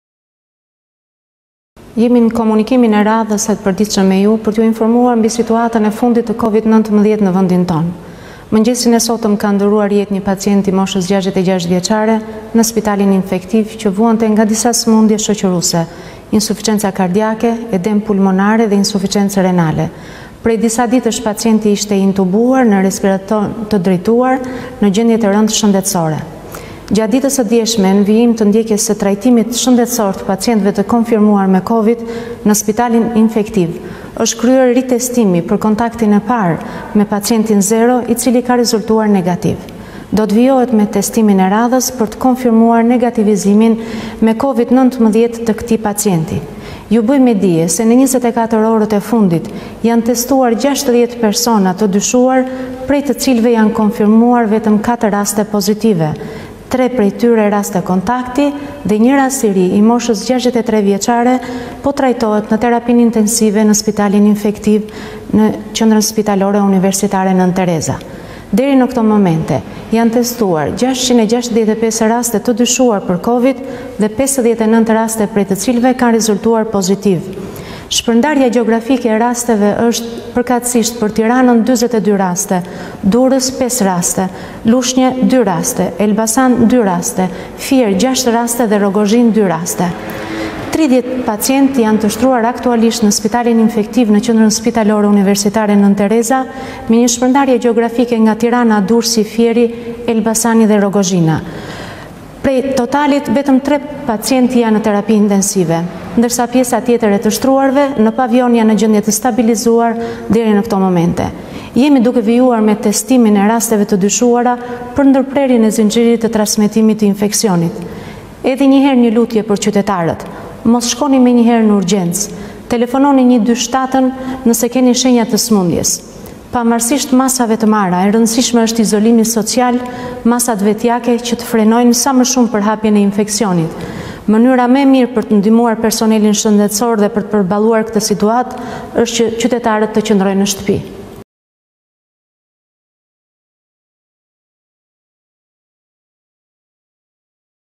Dosja.al- 59 të prekur nga koronavirusi në Shqipëri, 2 viktima, harta izolohet në Shqipërinë e Mesme
TIRANË- 4 raste të reja regjistrohen me koronavirus në Shqipëri, nga 60 persona që janë testuar në total përgjatë 24 orëve të fundit. Ministria e Shëndetësisë ka bërë përditësimin e të dhënave sot, ku ka bërë me dije, se nga 4 të infektuarit e fundit, 3 prej tyre janë raste kontakti, dhe vetëm një është një rast i ri, një pacient 66-vjeçar që po trajtohet në terapi intensive në spitalin infektiv në QSUT. 30 pacientë janë të shtruar në spitalin Infektiv (nga Tirana, Durrësi, Fieri, Elbasani  dhe Rrogozhina).

NJOFTIMI I PLOTE:

Ju informojmë për situatën e fundit të COVID-19 në Shqipëri. Mëngjesin e sotëm ka ndërruar jetë një pacient i moshës 66-vjecare në spitalin Infektiv, që vuante nga disa sëmundje shoqëruese: insuficenca kardiake, edemë pulmonarë dhe insuficencë renale. Prej disa ditësh pacienti ishte i intubuar (me respiracion të drejtuar) në gjendje të rëndë shëndetësore.

Gjatë ditës së djeshme, në vijim të ndjekjes dhe trajtimit shëndetësor të pacientëve të konfirmuar me COVID-19 në spitalin infektiv, është kryer ritestimi për kontaktin e parë të pacientit zero, i cili ka rezultuar negative. Do të vijohet me testin e radhës për të konfirmuar negativizimin nga COVID-19 për këtë pacient.
Ju bëj me dije se, në 24 orët e fundit janë testuar 60 persona të dyshuar, prej të cilëve janë konfirmuar 4 raste të reja me COVID-19. Tre janë raste kontakti në gjendje të mirë shëndetësore dhe një rast i ri, një pacient 66-vjeçar që po trajtohet në terapi intensive në spitalin infektiv në QSUT. Deri në këto momente janë testuar 665 raste të dyshuara dhe prej tyre janë të konfirmuar pozitivë me COVID-19, 59 raste:

Tiranë 42 raste
Durrës 5 raste
Lushnje 2 rast
Elbasan 2 raste
Fier 6 raste
Rrogozhinë 2 raste